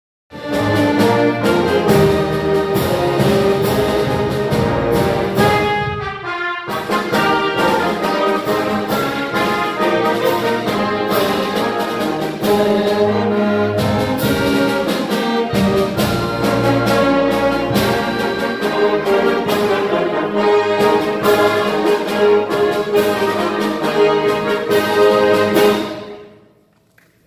LBHS spring concert 2018
Jannine is playing the french horn.